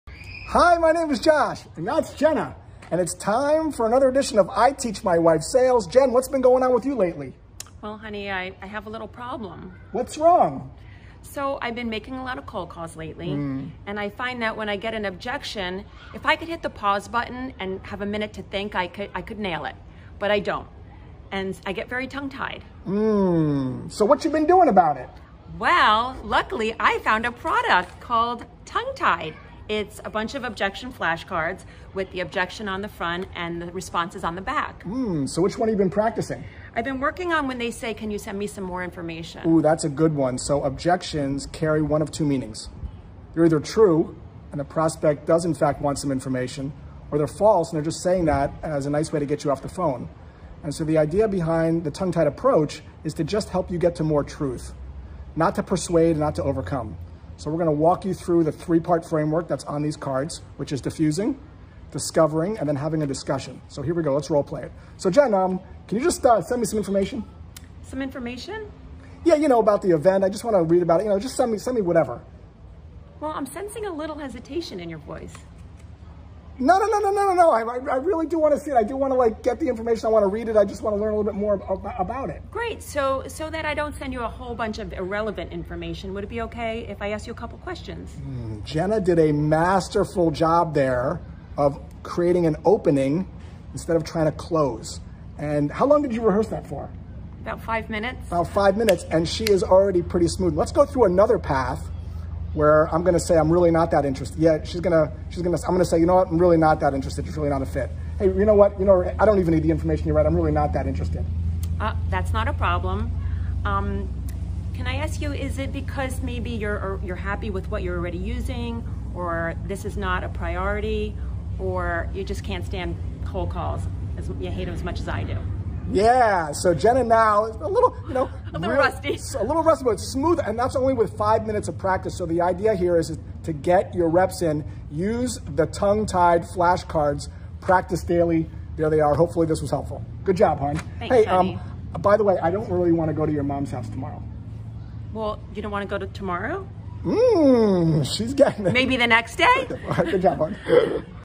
Objection Memorization - I Teach My Wife Sales
In this video, Jenna and I talk about objection memorization, and how you can better learn your response to sales objections through flash cards.